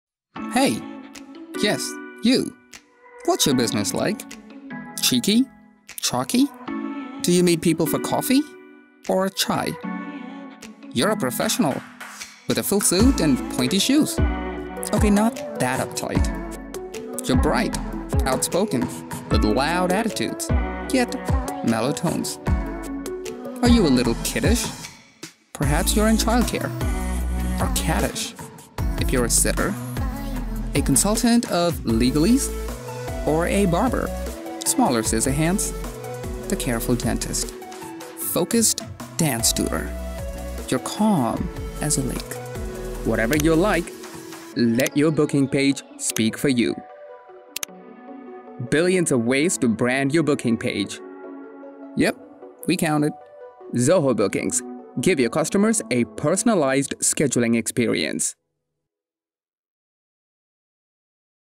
Zoho Bookings - For the best appointment scheduling pages that can represent your brand
Make your appointment booking page reflect your brand using features like color palette. Use the hex color picker to choose colors that match your brand colors and apply that on your booking page. Upload background images, logo and edit text to give the best scheduling experience.

Zoho Bookings is an appointment scheduling software for professional consultants and service providers to find the perfect time to meet with their customers.